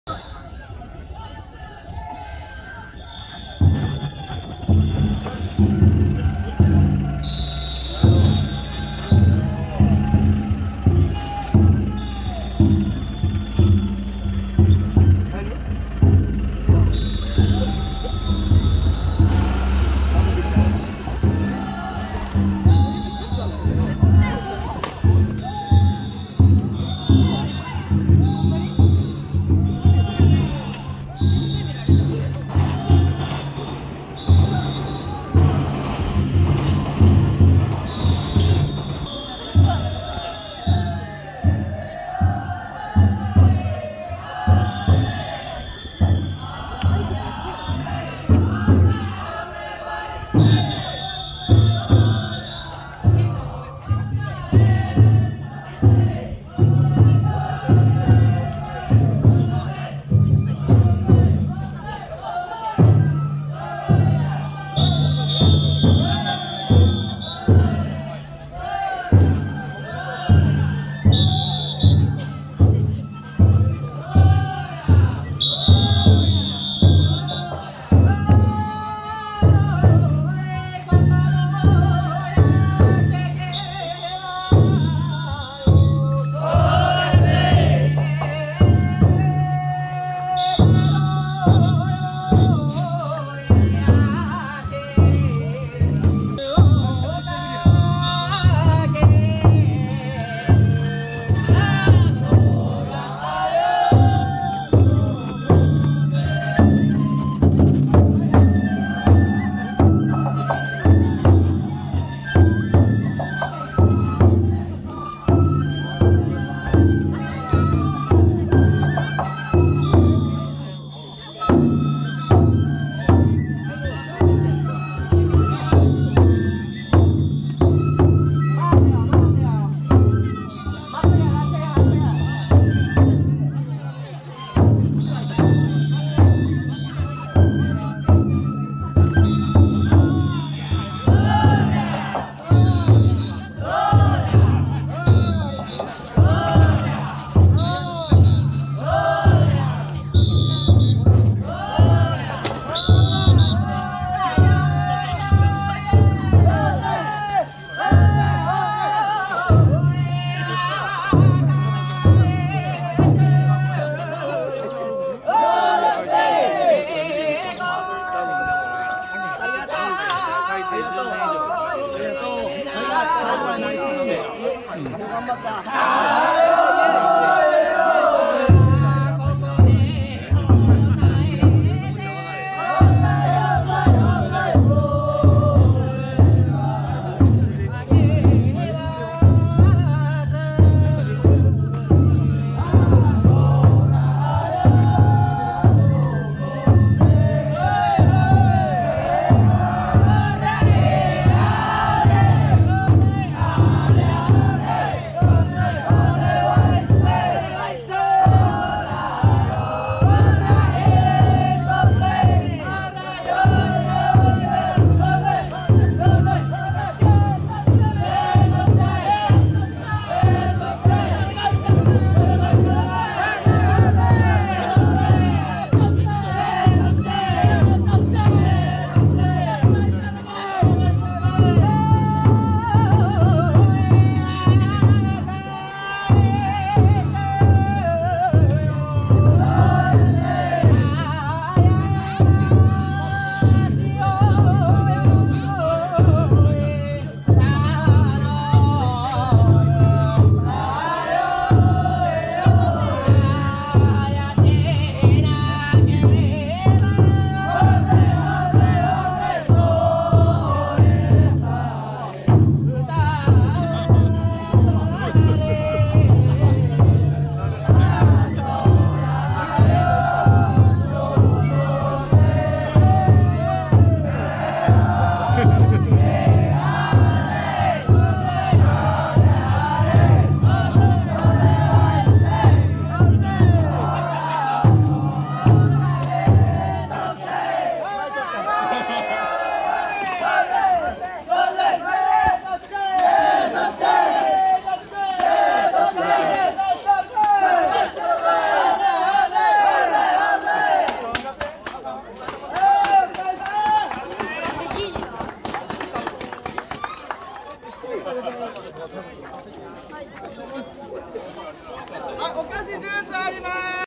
2023年 尾崎宮本町子どもイベント 子ども曳き(８Ｋ撮影)03
尾崎宮本町子どもイベント 子ども曳き
こどもの日ってことで子どもイベントでやぐらを出すとこを観にいってきました。
尾崎宮本町子どもイベントはやぐら部屋前で開催
やぐら部屋へ行くと既に子ども曳き中でしたので自転車で先回りしながら撮影しました。